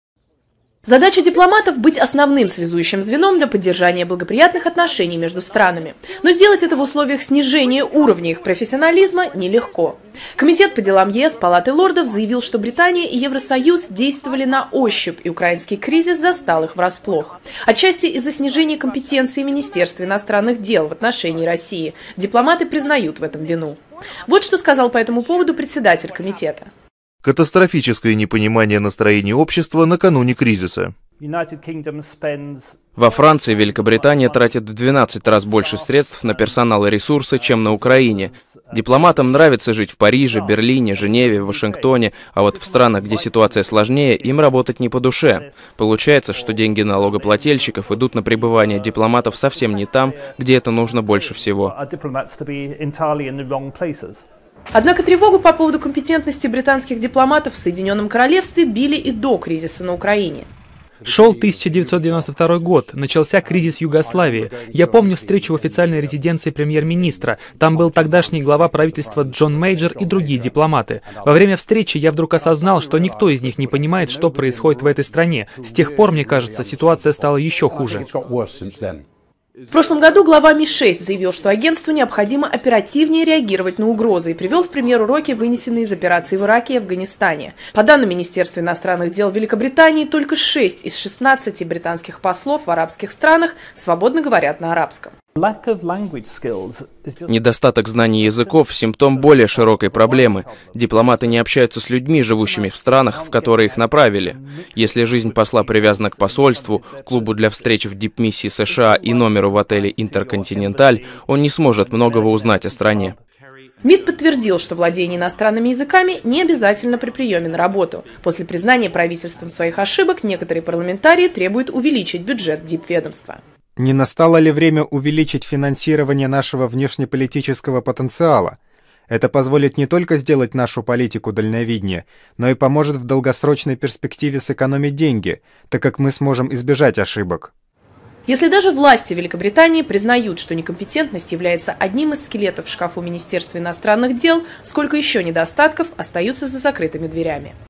24 04 2015 Россия сегодня новости Великобритания признала некомпетентность своих дипломатов 1
Самые свежие новости со всего мира.